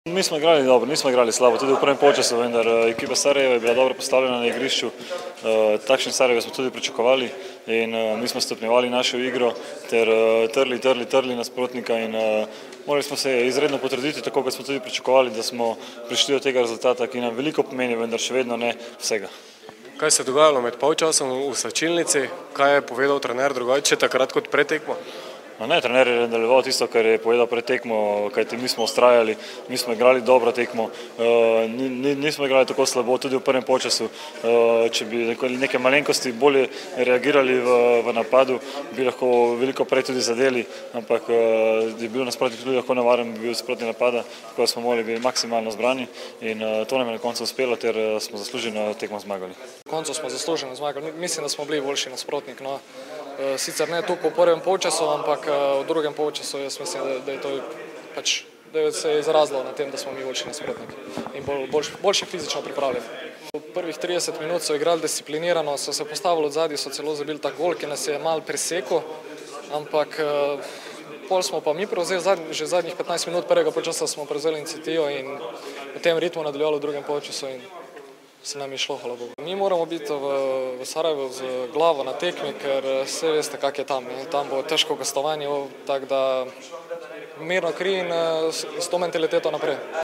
Berič blestel, ko je Maribor v drugem polčasu povozil Željezničar
Nogometaši Maribora so v drugem polčasu povozili Željezničar. Tekma se je končala s 4:1. Prvo ime tekme pa je bil z dvema zadetkoma in asistenco Robert Berič.